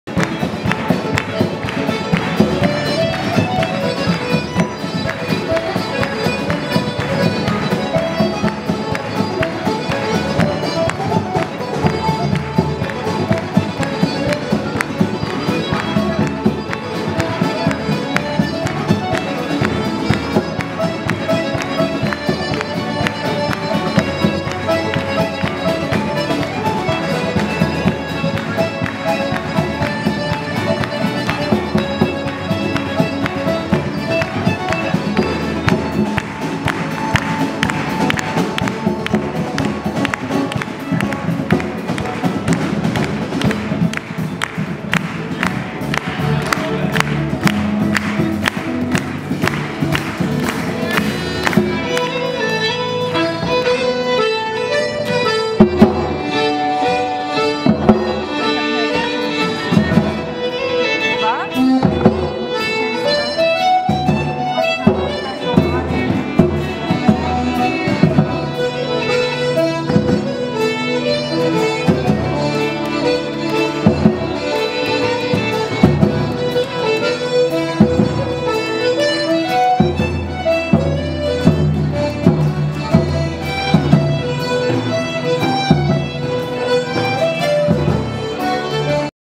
Newfoundland the band - the finale
A great performance by Newfoundland the band at their first ever visit to the province of Newfoundland Labrador. Their first stop was in St. John's for their 10 day tour.  Sure they will return again and again by the warm response by the sold out crowd